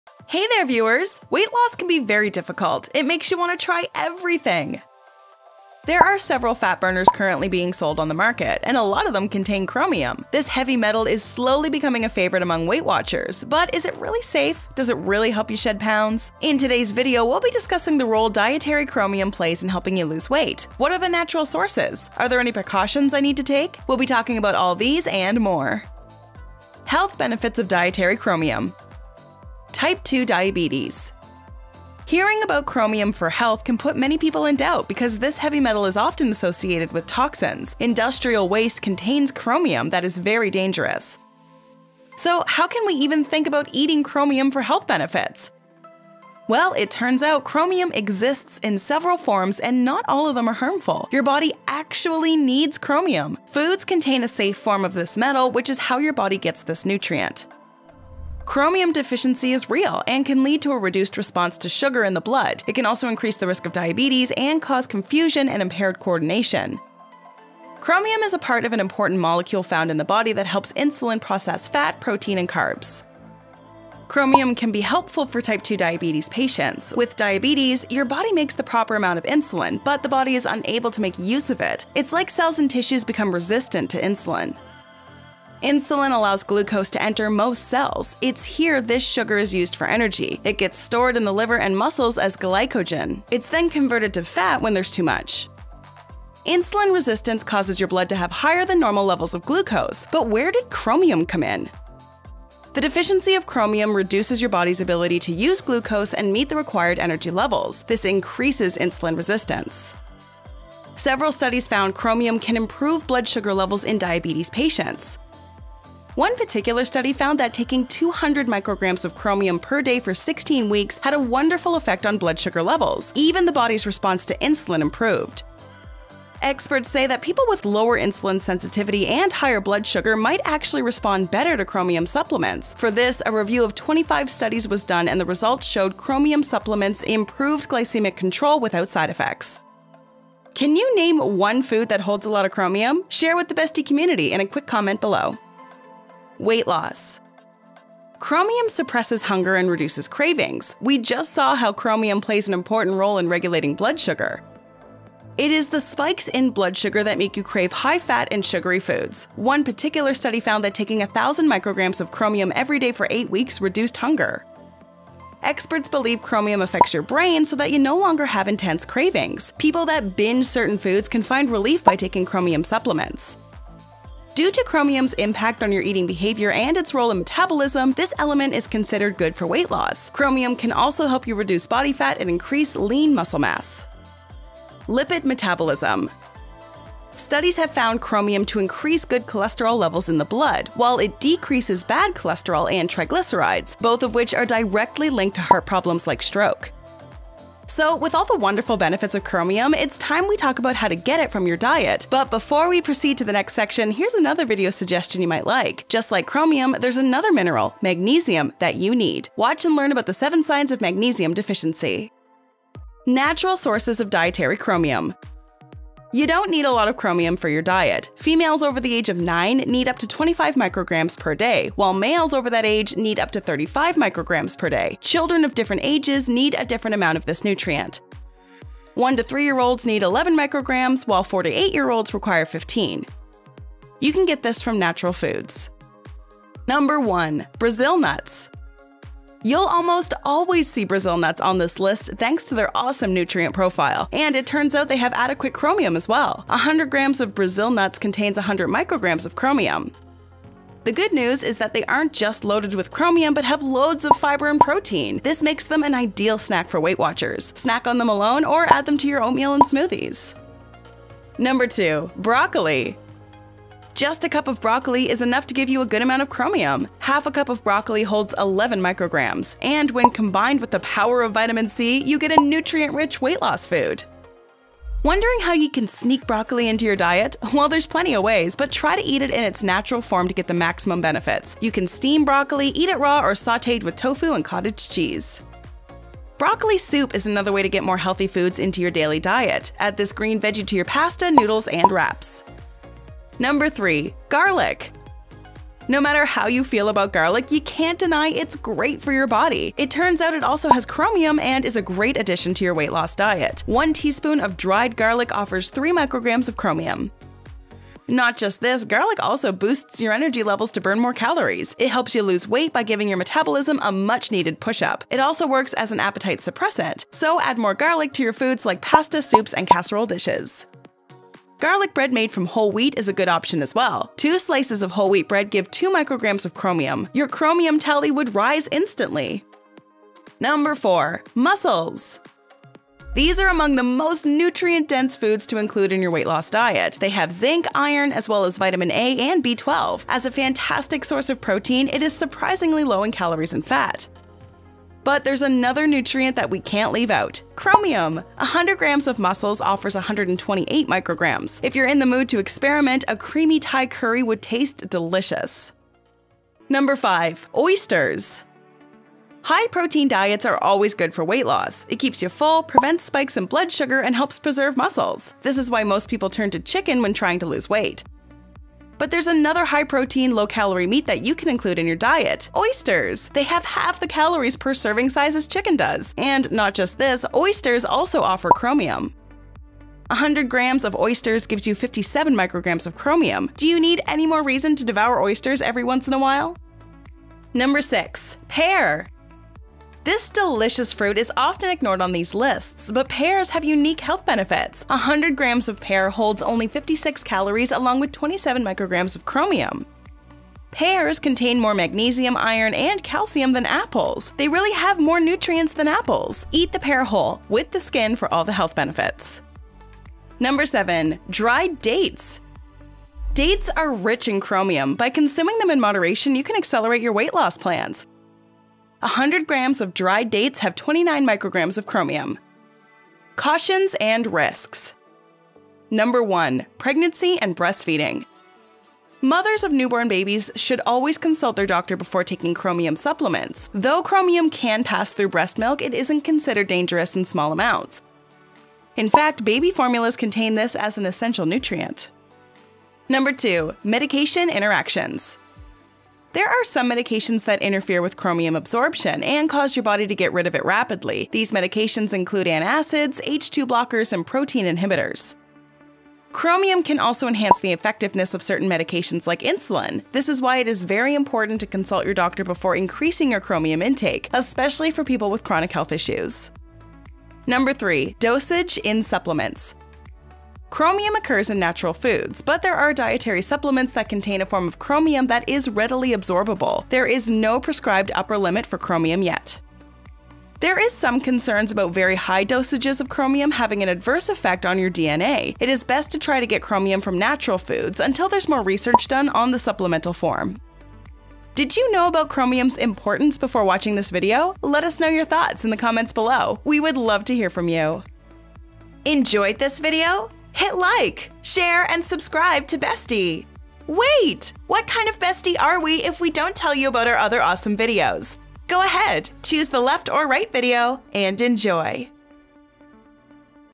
What Is Chromium And How Does It Help You Lose Weight?
In today’s video, we will be discussing the role dietary chromium plays in helping you lose weight. What are the natural sources? Are there any precautions I need to take? We'll be talking about all of these AND more.

Timestamps:
Intro - 0:00
Health Benefits of Dietary Chromium
1. Type 2 Diabetes - 00:27
2. Weight Loss - 02:25
3. Lipid Metabolism - 03:04

Natural Sources of Dietary Chromium
1. Brazil Nuts - 03:56
2. Broccoli - 04:21
3. Garlic - 04:53
4. Mussels - 05:30
5. Oysters - 05:56
6. Pear - 06:27
7. Dried Dates - 06:50

Cautions and Risks
1. Pregnancy and Breastfeeding - 07:03
2. Medication Interactions - 07:22
3. Dosage in Supplements - 07:47

Summary:
1. Type 2 Diabetes
Hearing about chromium for health can put many people in doubt because this heavy metal is often associated with toxins. Industrial waste contains chromium that is very dangerous.

2. Weight Loss
Chromium suppresses hunger and reduces cravings. We just saw how chromium plays an important role in regulating blood sugar.

3. Lipid Metabolism
Studies have found Chromium to increase good cholesterol levels in the blood, while it decreases bad cholesterol and triglycerides- both of which are directly linked to heart problems like stroke.

1. Brazil Nuts
You’ll almost always see Brazil nuts on this list thanks to their awesome nutrient profile. And it turns out they have adequate chromium as well. 100 grams of Brazil Nuts contain 100 micrograms of Chromium.

2. Broccoli
Just a cup of broccoli is enough to give you a good amount of Chromium. Half a cup of broccoli holds 11 micrograms. And when combined with the power of vitamin C, you get a nutrient-rich weight-loss food!

3. Garlic
No matter how you feel about garlic, you can’t deny it’s great for your body. It turns out it also has Chromium and is a great addition to your weight loss diet. 1 teaspoon of dried garlic offers 3 micrograms of Chromium.